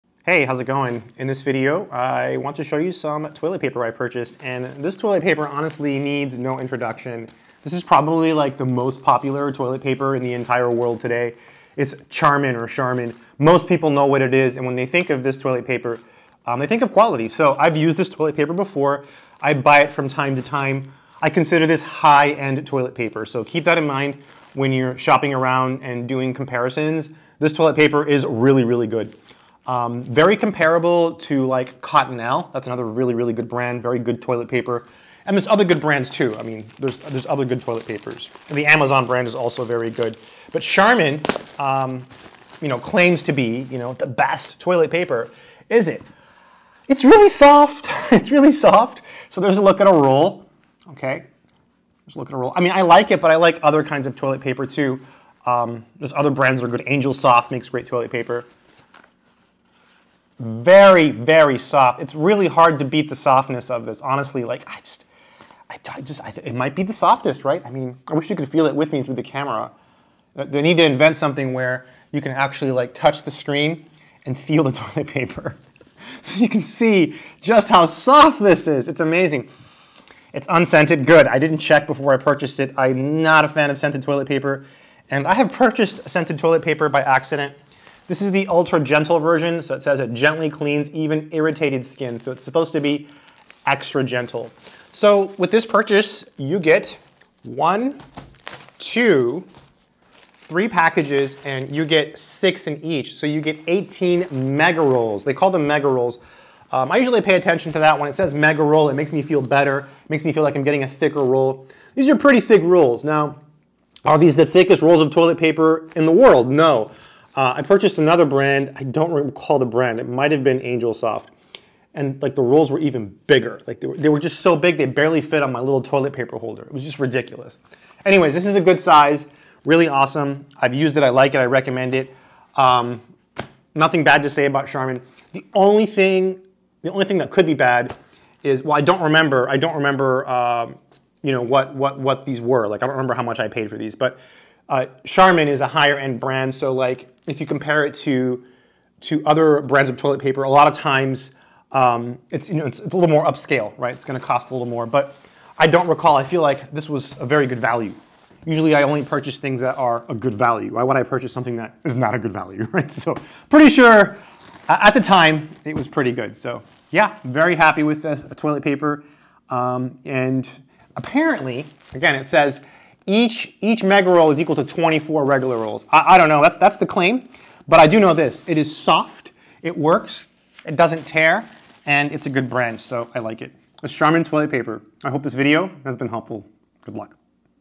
Charmin Ultra Gentle Toilet Paper
This is Charmin Ultra Gentle Toilet Paper. This a 3 pack and each pack comes with 6 mega rolls, so you get 18 rolls of super soft high quality toilet paper:)